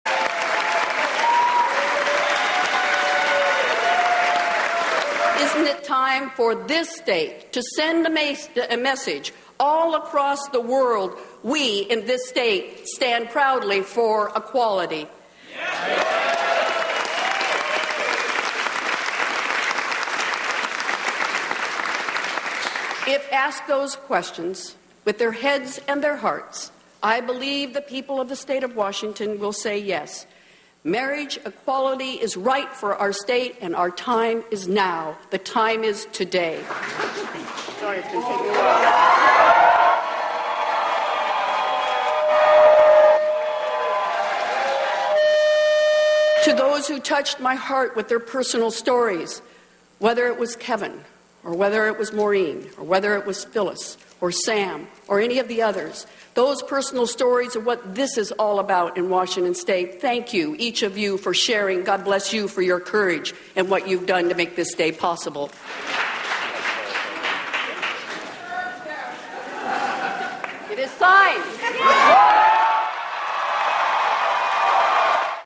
Gregoire: Marriage Equality Is Right for Wash.
Gov. Chris Gregoire handed gay rights advocates a major victory, signing into law a measure that legalizes same-sex marriage in Washington state, making it the seventh in the nation to allow gay and lesbian couples to wed. (Feb. 13)